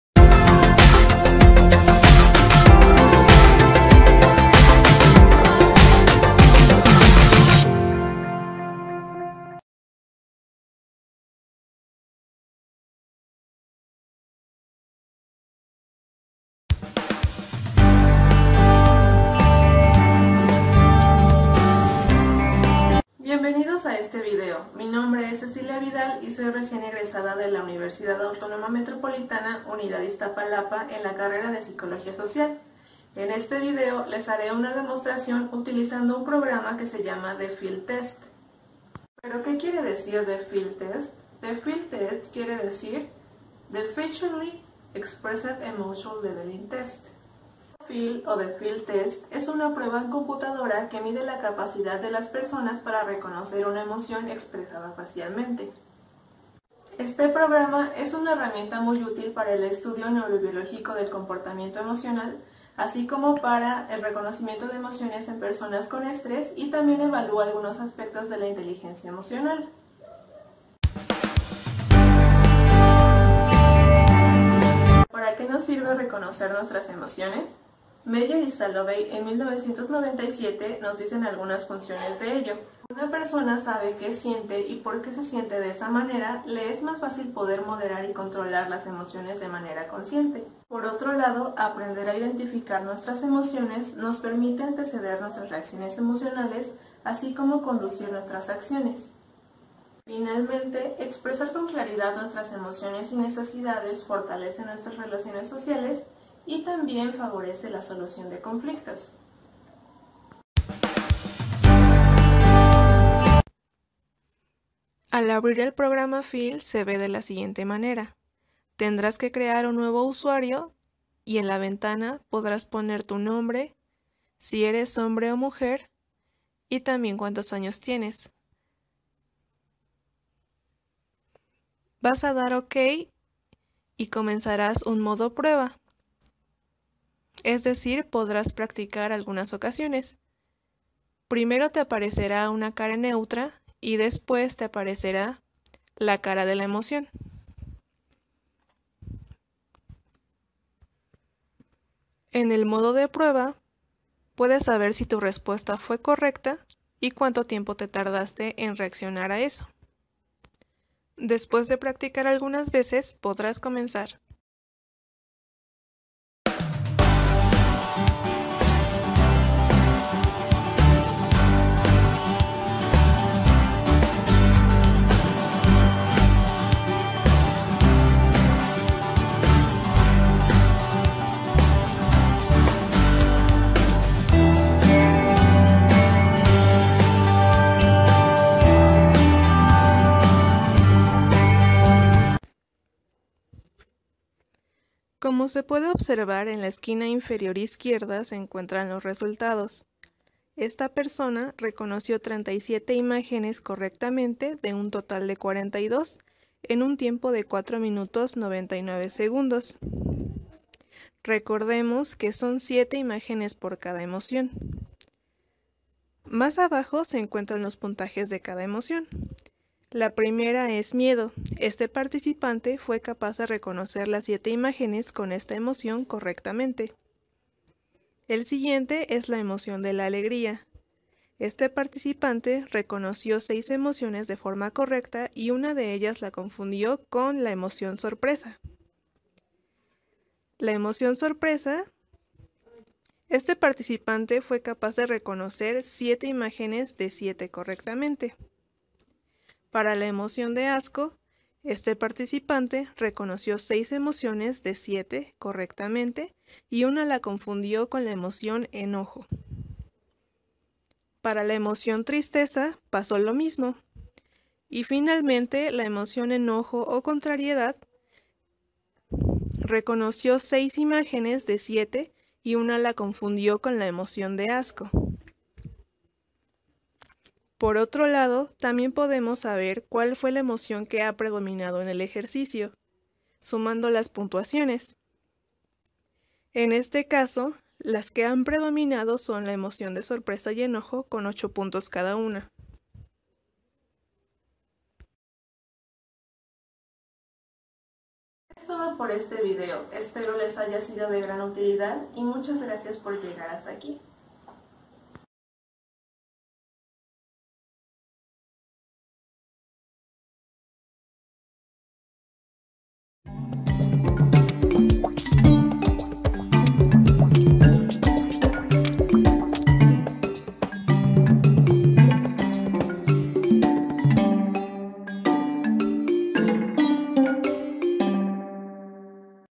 FEEL TEST. UNA FORMA DE MEDIR EL ESTRÉS.
STAND VIRTUAL "PROBLEMAS PSICOSOCIALES QUE AFECTAN A LOS CUIDADORES PRIMARIOS EN ÉPOCA DE COVID-19.
LUNES 23 DE NOVIEMBRE 2020
17:30 - 18:30 HORAS
Dr. Mario Carranza Aguilar
Dra. Marisol Pérez Ramos
Mtro. Omar Manjarrez Ibarra
Mtro. José Joel Vázquez Ortega
Dr. David Alberto Rodríguez Medina
Lic. Cecilia Concepción Vidal Romero
Lic. Marlen Silva Martínez
Departamento de Sociología, UAM-I
FaceBook Live